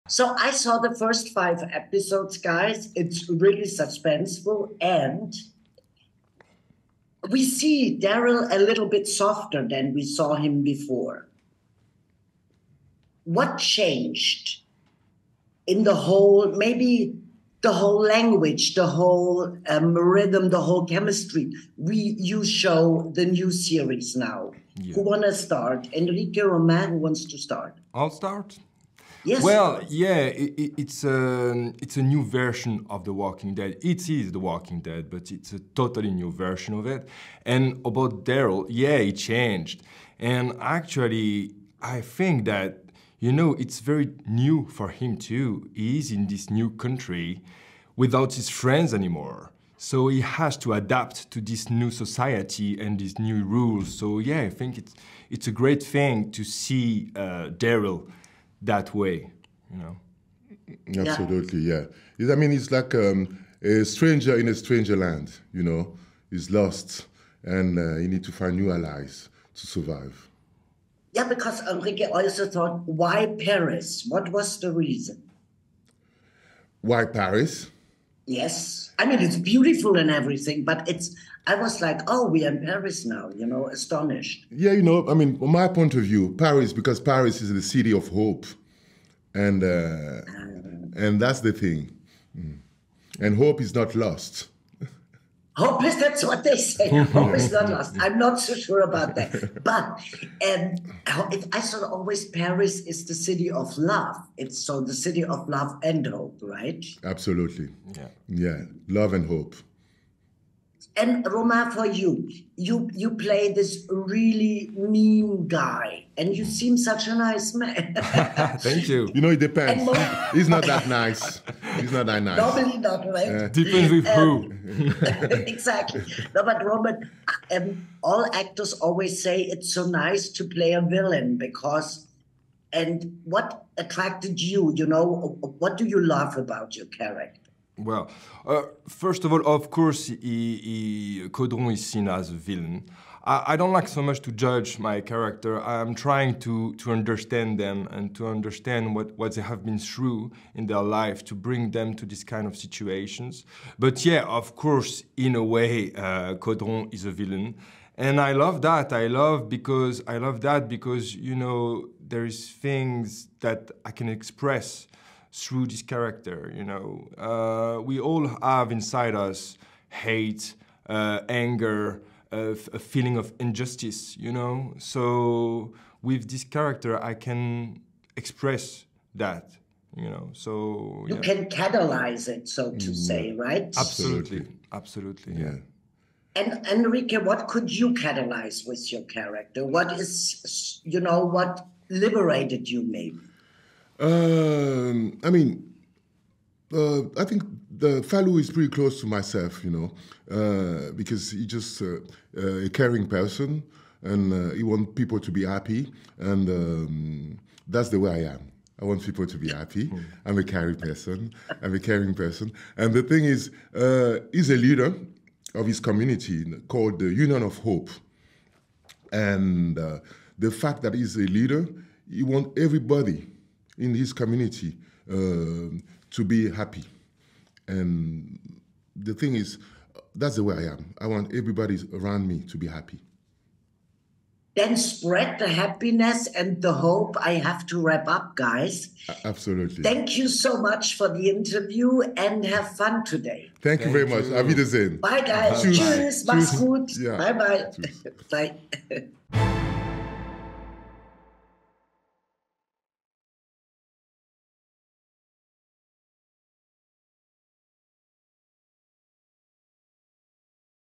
Eriq Ebouaney & Romain Levi THE WALKING DEAD: DARYL DIXON Interview (2023) // AMC
Hi there! Check out our interview with Eriq Ebouaney & Romain Levi about The Walking Dead: Daryl Dixon. :)